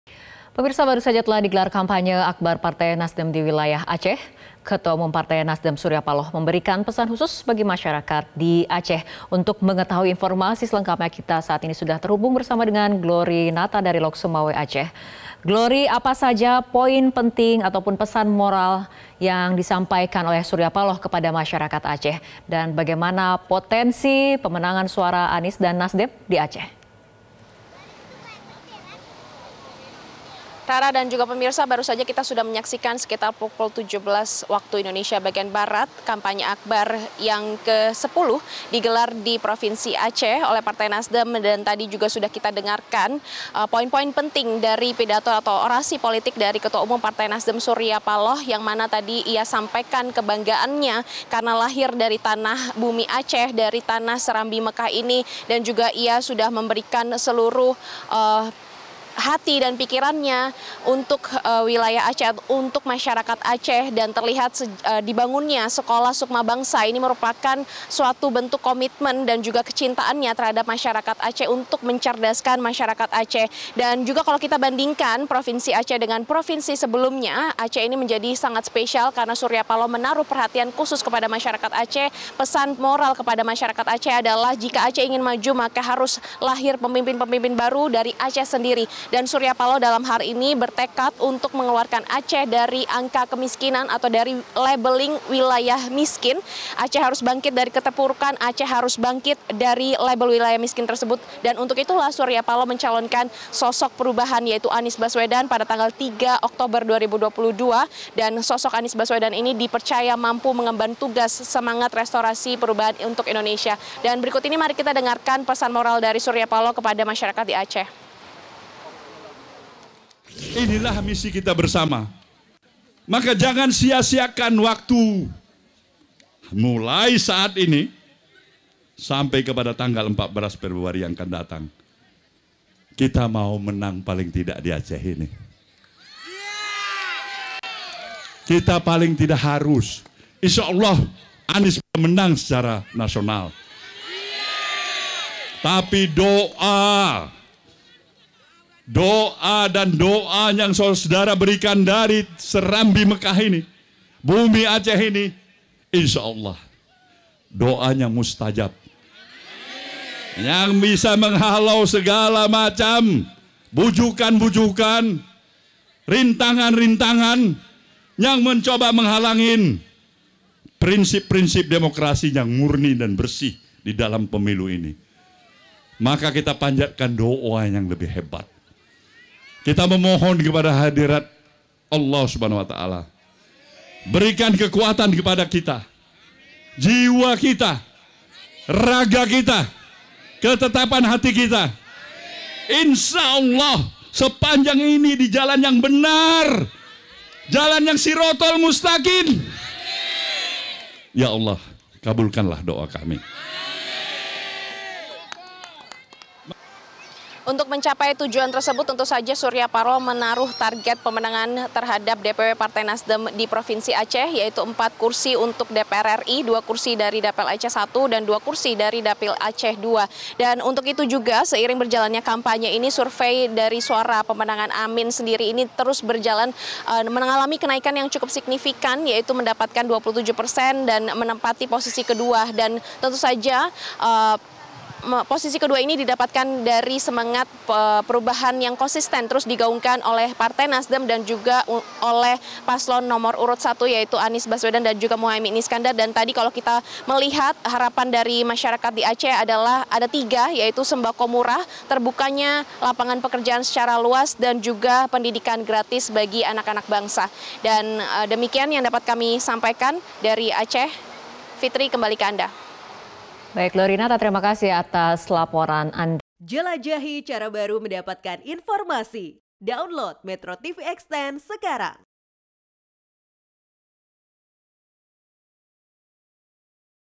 Tekad Surya Paloh Keluarkan Aceh dari Kemiskinan
MetroTV, Telah digelar Kampanye Akbar Partai NasDem di wilayah Aceh. Ketua Umum Partai NasDem, Surya Paloh memberikan pesan khusus bagi masyarakat di Aceh.